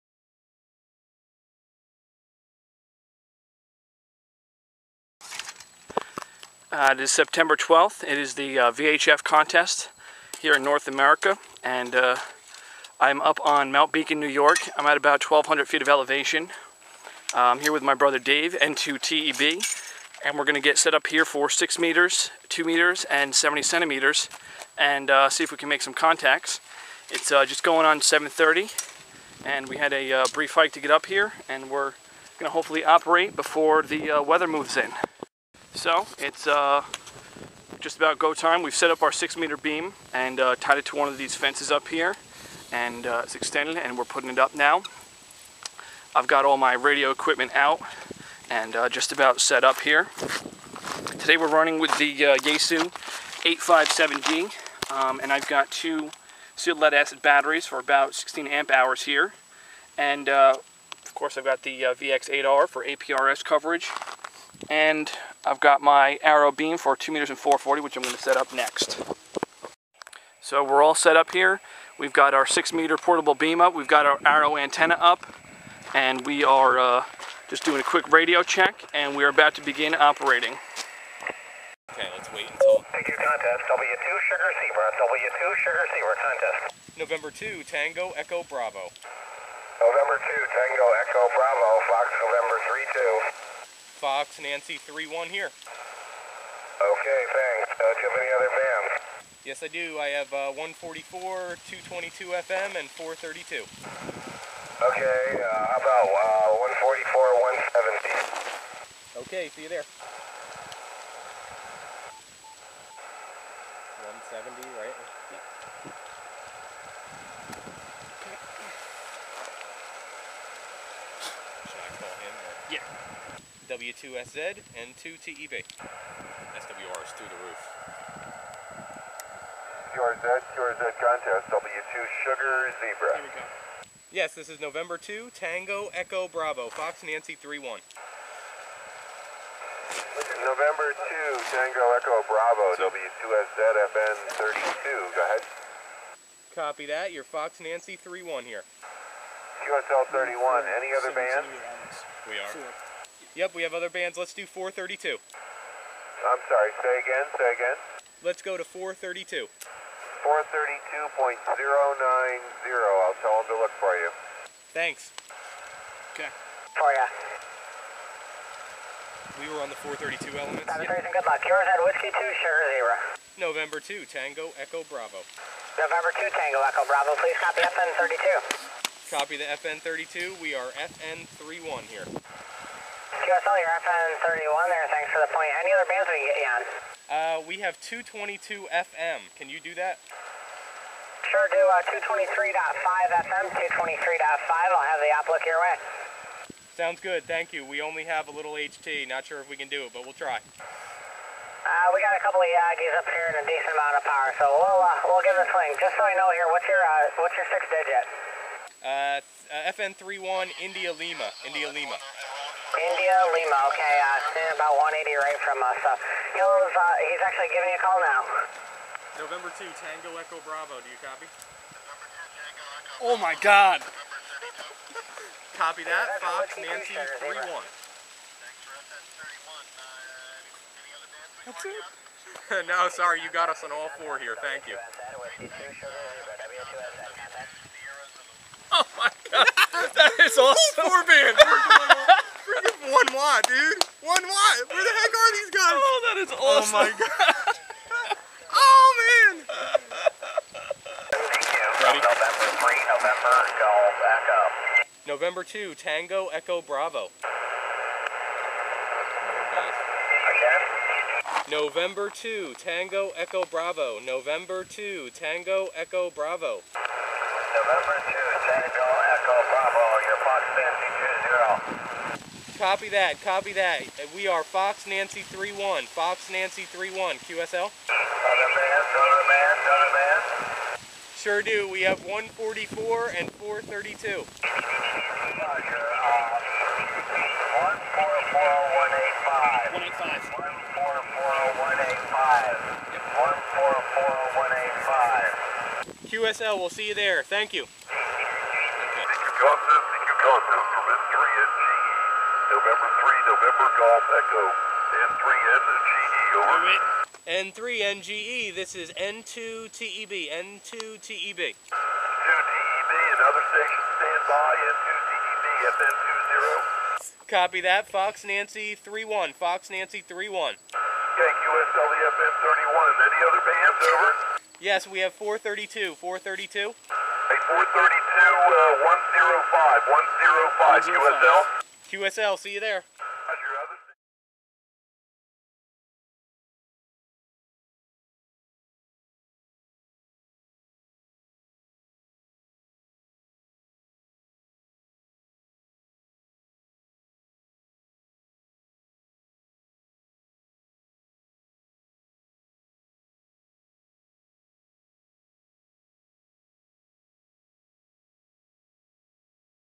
Sept 12th VHF Contest - K2FR / N2TEB Mountain top
Dave - N2TEB
Andrew - K2FR

We set out on an early morning hike to go mountain top portable for the VHF Contest going on.

The equipment worked great, and many contacts were had.  It was pretty freaking cool working people on 6m, then going to 2m, to 70cm, and upto 232